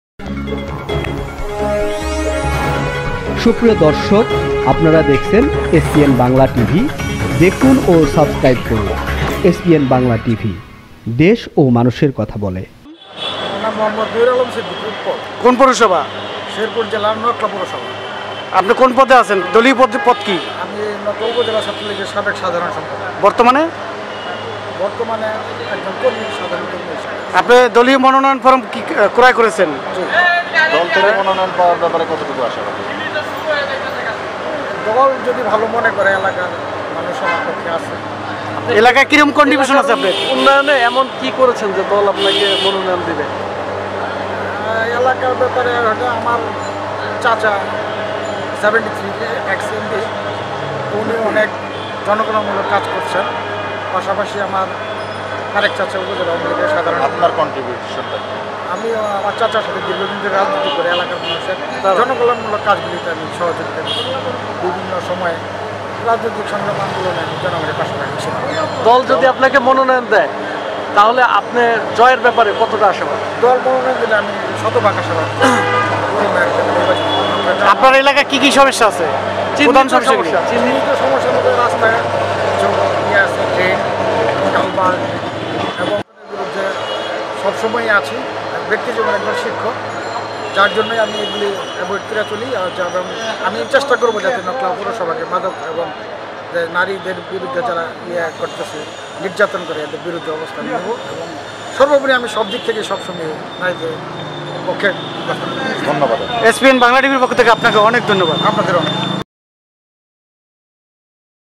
এসপিএন বাংলা টিভিকে সাক্ষাৎকার দেন শেরপুর জেলার নকলা পৌরসভার মেয়র পদপ্রার্থী নূরে আলম সিদ্দিকী উৎপল
এসপিএন বাংলা টিভি সংবাদ: আজ ২০ ডিসেম্বর ২০২০, পৌরসভার মেয়াদ উত্তিন্ন হওয়ায় তৃতীয় ধাপে পৌরসভার নির্বাচন অনুষ্ঠিত হতে যাচ্ছে। পৌরসভা নির্বাচন দলীয় প্রতীকে হওয়ার কারণে বাংলাদেশ আওয়ামী লীগ ধানমন্ডী ৩/এ সভাপতির কার্যালয় থেকে প্রার্থীরা মনোনয়ন ফরম সংগ্রহ ও জমা করে  এসপিএন বাংলা টিভিকে সাক্ষাৎকার দেন শেরপুর জেলার নকলা পৌরসভার মেয়র পদপ্রার্থী নূরে আলম সিদ্দিকী উৎপল। পরিচালনায়: আজিজুল হক মিন্টু যোগাযোগ:০১৭১১-১৪৩৩৪২।